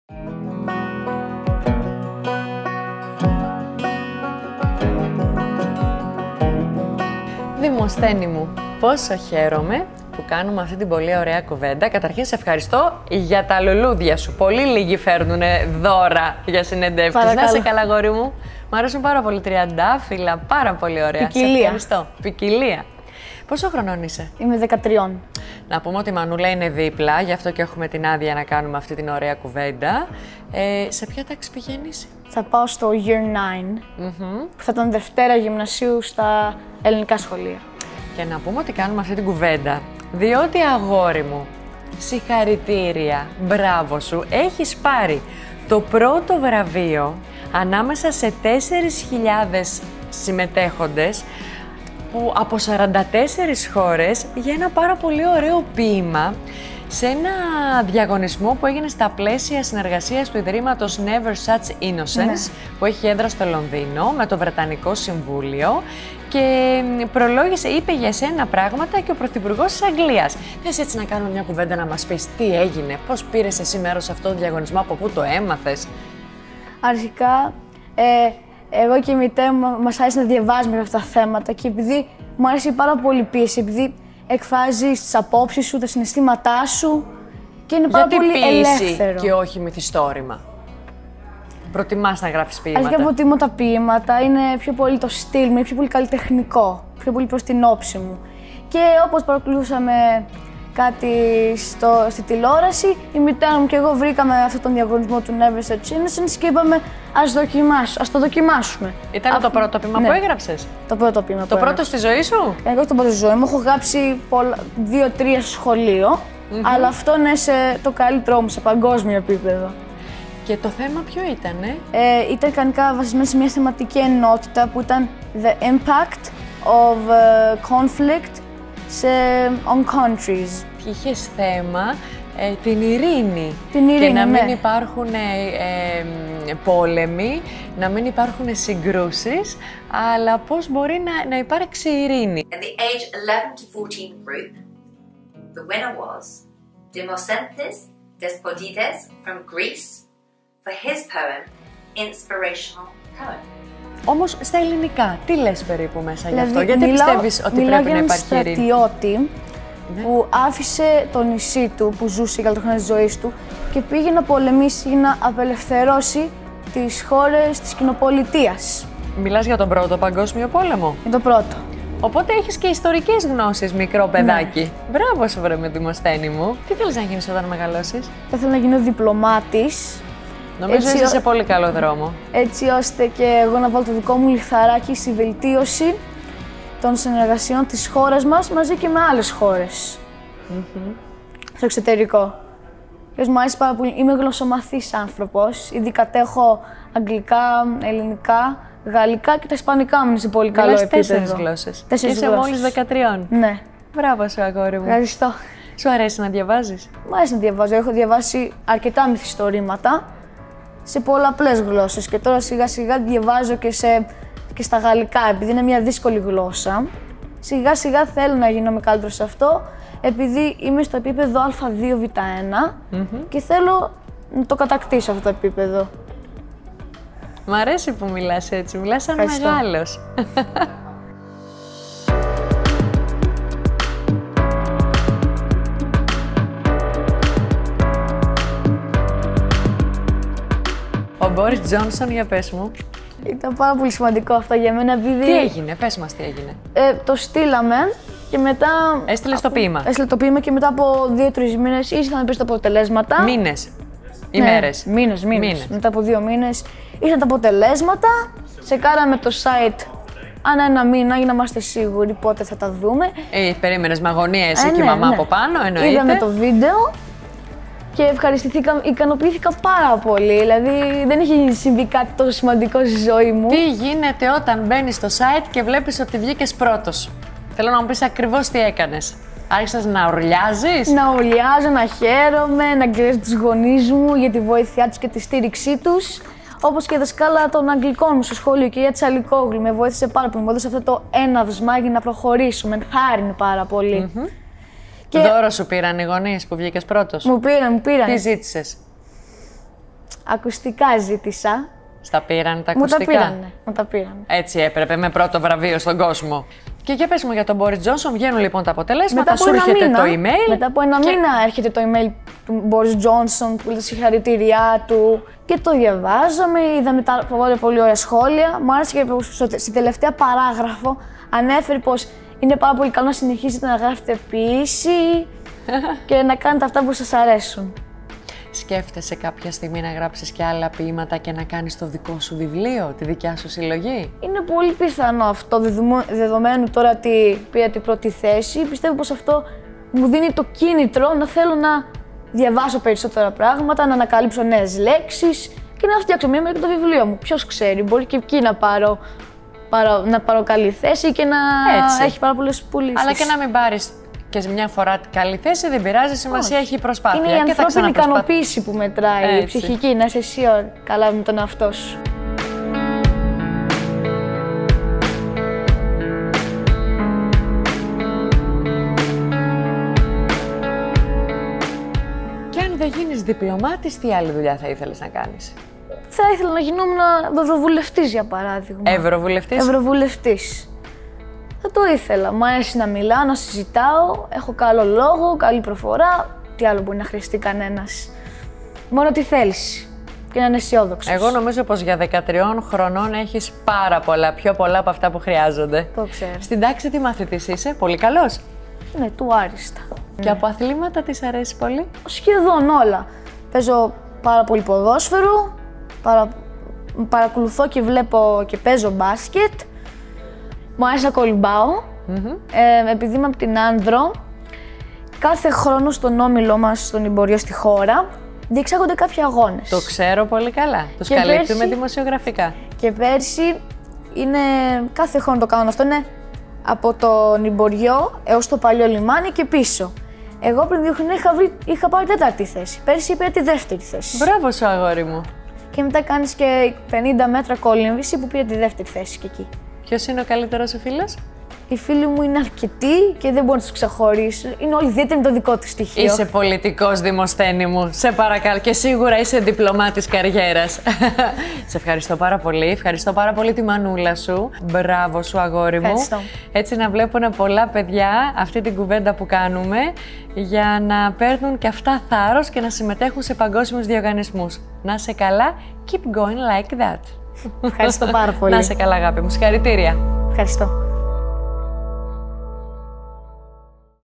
Special Edition: Ένας ποιητής 13 ετών
Πόσο υπερήφανη είναι η μητέρα του μικρού Δημοσθένη που κατέκτησε το πρώτο βραβείο ποίησης σε παγκόσμιο διαγωνισμό του ιδρύματος “never such innocence” και πόσο υπερήφανοι είμαστε εμείς σαν Έλληνες για αυτή την διάκριση! Ο Δημοσθένης μέσω του ποιήματος του κατέκτησε την πρώτη θέση στον παγκόσμιο διαγωνισμό ποίησης με το ποίημα του που έχει τίτλο “Ποίημα Έμπνευσης”.

Η κριτική επιτροπή εντυπωσιάστηκε και τον επέλεξε ανάμεσα σε 4.000 παιδιά από 44 χώρες ανά τον κόσμο. Πρόκειται για ένα ποίημα υψηλού επιπέδου. Τις εργασίες των παιδιών προλόγισε ο Πρωθυπουργός της Μ. Βρεταννίας κ. Boris Johnson.

Συγχαρητήρια Δημοσθένη! Sky is the limit!